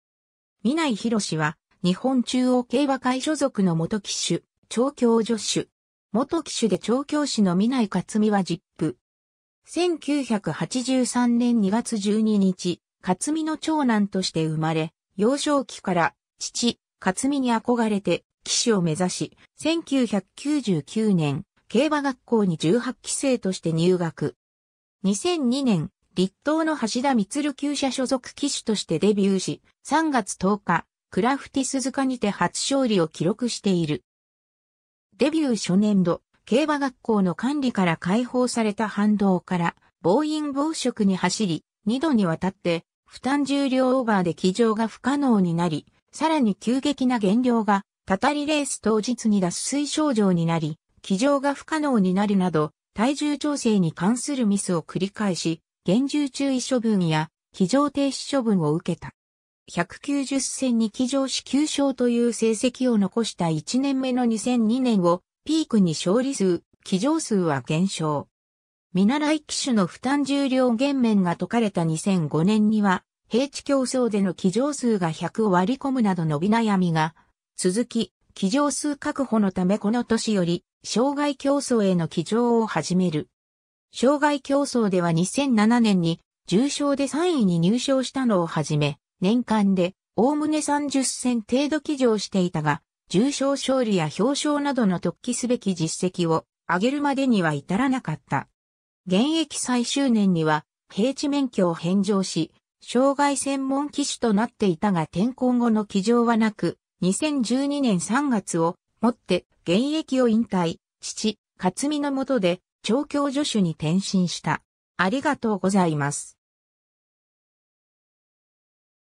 南井大志
南井 大志（みない ひろし、1983年2月12日 -）は日本中央競馬会 (JRA) 所属の元騎手・調教助手。
元騎手で調教師の南井克巳は実父。
 1983年2月12日、克巳の長男として生まれ、幼少期から父・克巳に憧れて騎手を目指し、1999年、競馬学校に18期生として入学。
 2002年、栗東の橋田満厩舎所属騎手としてデビューし、3月10日クラフティスズカにて初勝利を記録している。
 デビュー初年度、競馬学校の管理から解放された反動から暴飲暴食に走り、2度にわたって負担重量オーバーで騎乗が不可能になり、さらに急激な減量がたたりレース当日に脱水症状になり騎乗が不可能になるなど体重調整に関するミスを繰り返し、厳重注意処分や騎乗停止処分を受けた。
 190戦に騎乗し9勝という成績を残した1年目の2002年をピークに勝利数・騎乗数は減少。
見習騎手の負担重量減免が解かれた2005年には平地競走での騎乗数が100を割込むなど伸び悩みが続き、騎乗数確保のためこの年より障害競走への騎乗を始める。
 障害競走では2007年に重賞で3位に入賞したのをはじめ、年間でおおむね30戦程度騎乗していたが、重賞勝利や表彰などの特記すべき実績を挙げるまでには至らなかった。
現役最終年には平地免許を返上し障害専門騎手となっていたが転向後の騎乗はなく、2012年3月をもって現役を引退、父・克巳のもとで調教助手に転身した。